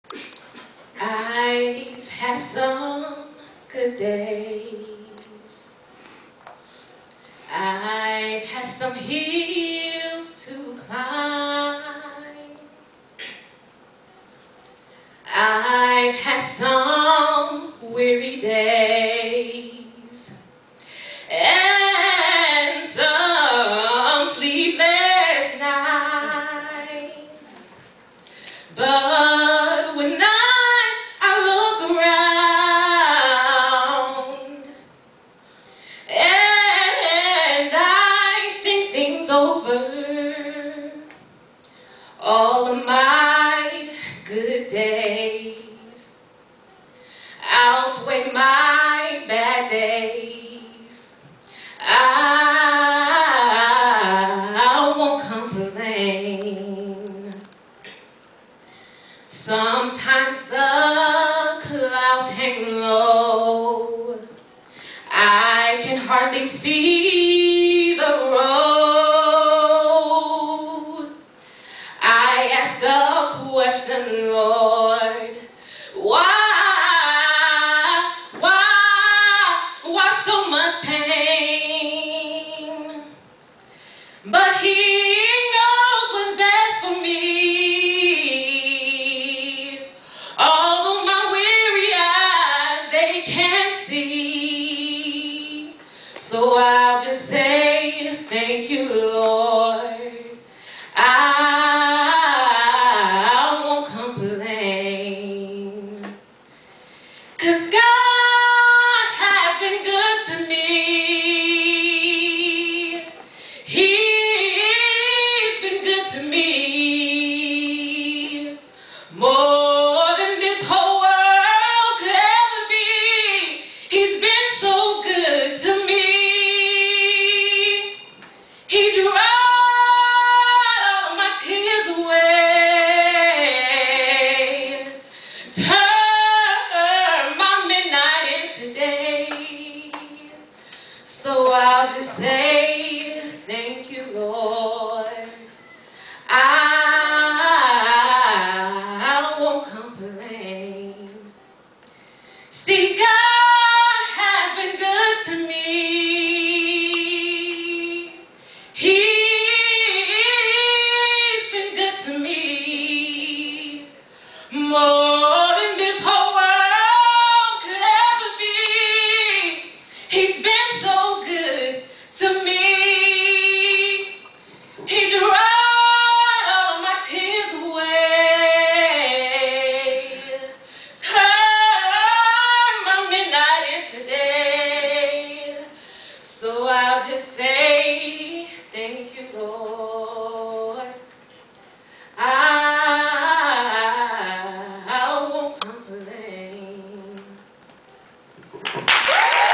BUSM Open Mic Night - Erica Perry - Religious Song
Erica Perry (BUSM M2) performs at Boston University School of Medicine's Open Mic Night 12/05/2011